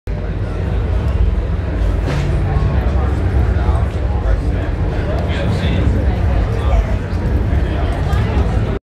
Gotj 2011 Merch Tent
Look at all that merch. If only I had won the lotto. MMFCL